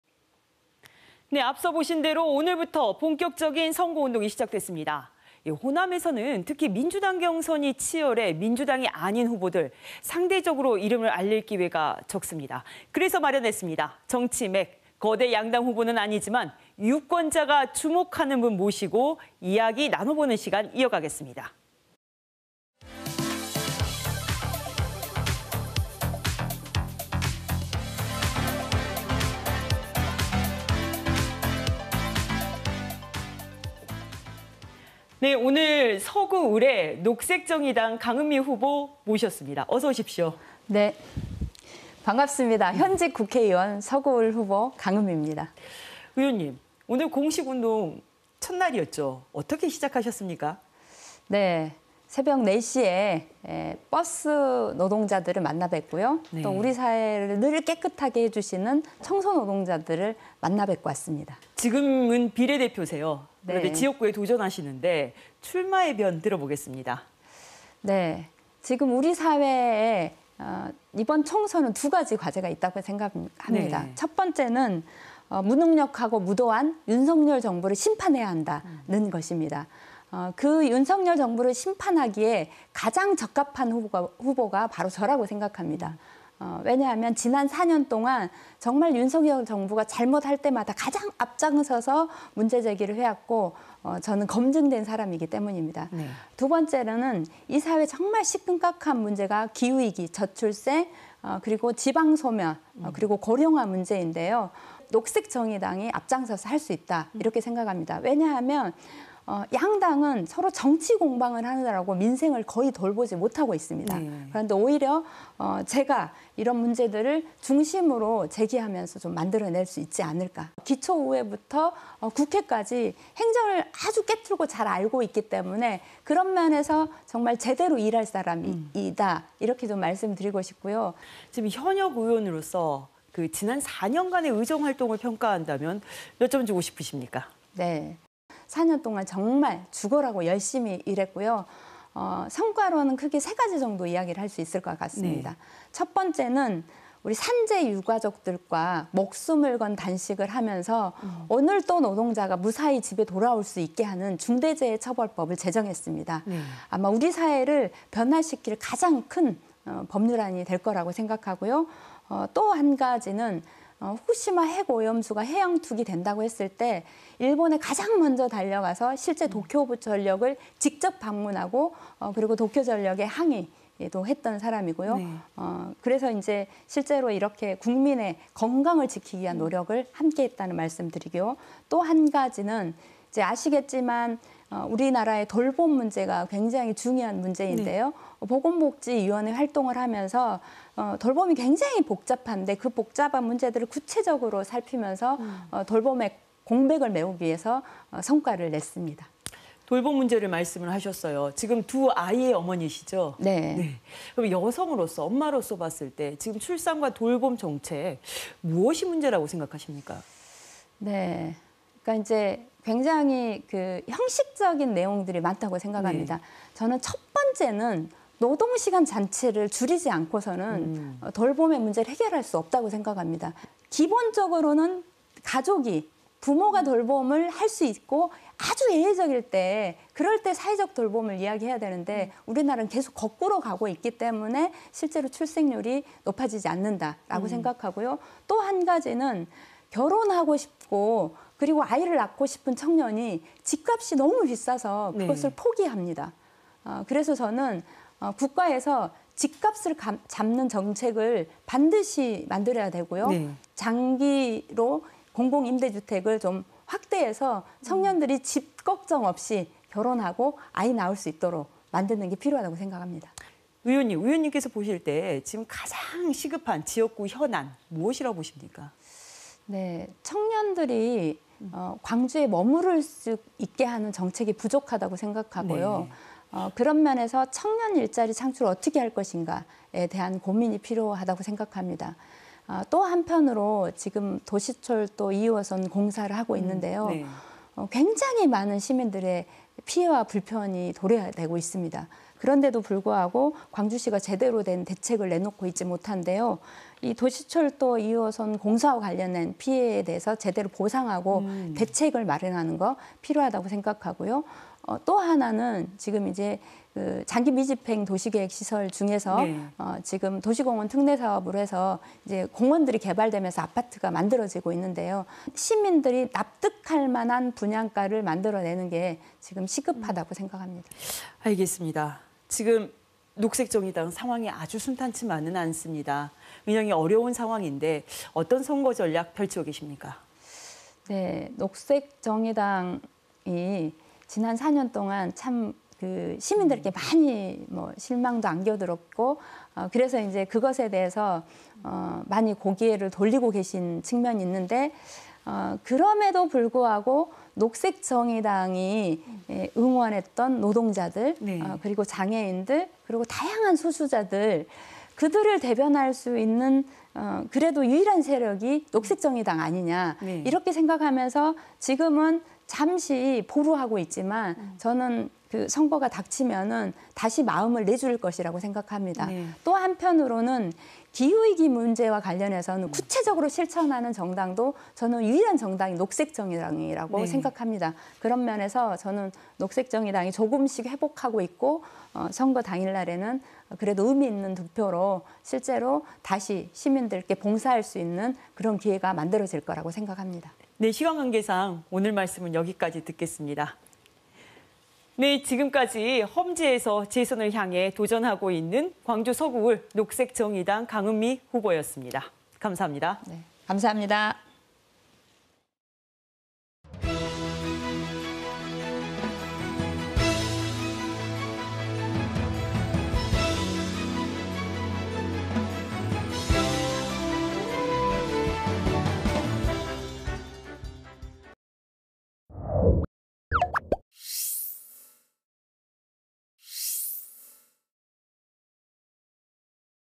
[총선/정치맥] ‘광주 서구을’ 녹색정의당 강은미 후보를 만나다 / KBS 2024.03.28.
[앵커]

 앞서 보신 대로 오늘부터 본격적인 선거운동이 시작됐습니다.

 호남에서는 특히 민주당 경선이 치열해 민주당이 아닌 후보들 상대적으로 이름을 알릴 기회가 적습니다.

 그래서 마련했습니다.

 '정치맥' 거대 양당 후보는 아니지만, 유권자가 주목하는 분 모시고 이야기 나눠보는 시간 이어가겠습니다.

 오늘 서구을에 녹색정의당 강은미 후보 모셨습니다.

 어서 오십시오.

 의원님 오늘 공식 운동 첫날이었죠.

 어떻게 시작하셨습니까?

 [답변]

 네 새벽 4시에 버스 노동자들을 만나 뵙고요.

 또 우리 사회를 늘 깨끗하게 해주시는 청소 노동자들을 만나 뵙고 왔습니다.

 [앵커]

 지금은 비례대표세요.

 그런데 지역구에 도전하시는데 출마의 변 들어보겠습니다.

 [답변]

 네 지금 우리 사회에 이번 총선은 두 가지 과제가 있다고 생각합니다.

 첫 번째는 무능력하고 무도한 윤석열 정부를 심판해야 한다는 것입니다.

 그 윤석열 정부를 심판하기에 가장 적합한 후보가 바로 저라고 생각합니다.

 왜냐하면 지난 4년 동안 정말 윤석열 정부가 잘못할 때마다 가장 앞장서서 문제제기를 해왔고 저는 검증된 사람이기 때문입니다.

 두 번째로는 이 사회에 정말 심각한 문제가 기후위기, 저출생 그리고 지방 소멸 그리고 고령화 문제인데요.

 녹색 정의당이 앞장서서 할 수 있다 이렇게 생각합니다.

 왜냐하면 양당은 서로 정치 공방을 하느라고 민생을 거의 돌보지 못하고 있습니다.

 그런데 오히려 제가 이런 문제들을 중심으로 제기하면서 만들어낼 수 있지 않을까, 기초의회부터 국회까지 행정을 아주 꿰뚫고 잘 알고 있기 때문에 그런 면에서 정말 제대로 일할 사람이다 이렇게 말씀드리고 싶습니다.

 [앵커]

 지금 현역 의원으로서 지난 4년간의 의정활동을 평가한다면 몇 점 주고 싶으십니까?

 [답변]

 네, 4년 동안 정말 죽어라고 열심히 일했고요.

 성과로는 크게 세 가지 정도 이야기를 할 수 있을 것 같습니다.

 첫 번째는 우리 산재 유가족들과 목숨을 건 단식을 하면서 오늘도 노동자가 무사히 집에 돌아올 수 있게 하는 중대재해처벌법을 제정했습니다.

 아마 우리 사회를 변화시킬 가장 큰 법률안이 될 거라고 생각하고요.

 또 한 가지는 후쿠시마 핵오염수가 해양 투기 된다고 했을 때 일본에 가장 먼저 달려가서 실제 도쿄 전력을 직접 방문하고 그리고 도쿄 전력에 항의 했던 사람이고요.

그래서 이제 실제로 이렇게 국민의 건강을 지키기 위한 노력을 함께 했다는 말씀드리고요.

 또 한 가지는 이제 아시겠지만 우리나라의 돌봄 문제가 굉장히 중요한 문제인데요.

 보건복지위원회 활동을 하면서 돌봄이 굉장히 복잡한데 그 복잡한 문제들을 구체적으로 살피면서 돌봄의 공백을 메우기 위해서 성과를 냈습니다.

 [앵커]

 돌봄 문제를 말씀을 하셨어요.

 지금 두 아이의 어머니시죠?

 그러면 여성으로서 엄마로서 봤을 때 지금 출산과 돌봄 정책, 무엇이 문제라고 생각하십니까?

 [답변]

 그러니까 이제 굉장히 형식적인 내용들이 많다고 생각합니다.

 저는 첫 번째는 노동시간 자체를 줄이지 않고서는 돌봄의 문제를 해결할 수 없다고 생각합니다.

 기본적으로는 가족이, 부모가 돌봄을 할 수 있고 아주 예외적일 때 그럴 때 사회적 돌봄을 이야기해야 되는데 우리나라는 계속 거꾸로 가고 있기 때문에 실제로 출생률이 높아지지 않는다라고 생각하고요.

 또 한 가지는 결혼하고 싶고 그리고 아이를 낳고 싶은 청년이 집값이 너무 비싸서 그것을 포기합니다.

 그래서 저는 국가에서 집값을 잡는 정책을 반드시 만들어야 되고요.

 장기로 공공임대주택을 좀 확대해서 청년들이 집 걱정 없이 결혼하고 아이 낳을 수 있도록 만드는 게 필요하다고 생각합니다.

 [앵커]

 의원님, 의원님께서 보실 때 지금 가장 시급한 지역구 현안 무엇이라고 보십니까?

 [답변]

 청년들이 광주에 머무를 수 있게 하는 정책이 부족하다고 생각하고요.

 그런 면에서 청년 일자리 창출을 어떻게 할 것인가에 대한 고민이 필요하다고 생각합니다.

 또 한편으로 지금 도시철도 2호선 공사를 하고 있는데요.

 굉장히 많은 시민들의 피해와 불편이 도래되고 있습니다.

 그런데도 불구하고 광주시가 제대로 된 대책을 내놓고 있지 못한데요.

 도시철도 2호선 공사와 관련된 피해에 대해서 제대로 보상하고 대책을 마련하는 것 필요하다고 생각하고요.

 또 하나는 지금 이제 장기 미집행 도시계획시설 중에서 지금 도시공원 특례사업으로 해서 이제 공원들이 개발되면서 아파트가 만들어지고 있는데요.

 시민들이 납득할 만한 분양가를 만들어내는 게 지금 시급하다고 생각합니다.

 [앵커]

 알겠습니다.

 지금 녹색정의당 상황이 아주 순탄치만은 않습니다.

 운영이 어려운 상황인데 어떤 선거 전략 펼치고 계십니까?

 [답변]

 녹색 정의당이 지난 4년 동안 참 시민들에게 많이 실망도 안겨드렸고 그래서 이제 그것에 대해서 많이 고개를 돌리고 계신 측면이 있는데 그럼에도 불구하고 녹색 정의당이 응원했던 노동자들 그리고 장애인들 그리고 다양한 소수자들 그들을 대변할 수 있는 그래도 유일한 세력이 녹색 정의당 아니냐 이렇게 생각하면서 지금은 잠시 보류하고 있지만 저는 선거가 닥치면 다시 마음을 내줄 것이라고 생각합니다.

 또 한편으로는 기후위기 문제와 관련해서는 구체적으로 실천하는 정당도 저는 유일한 정당이 녹색 정의당이라고 생각합니다.

 그런 면에서 저는 녹색 정의당이 조금씩 회복하고 있고 선거 당일에는 그래도 의미 있는 득표로 실제로 다시 시민들께 봉사할 수 있는 그런 기회가 만들어질 거라고 생각합니다.

 [앵커]

 시간관계상 오늘 말씀은 여기까지 듣겠습니다.

 지금까지 험지에서 재선을 향해 도전하고 있는 광주 서구을 녹색정의당 강은미 후보였습니다.

 감사합니다.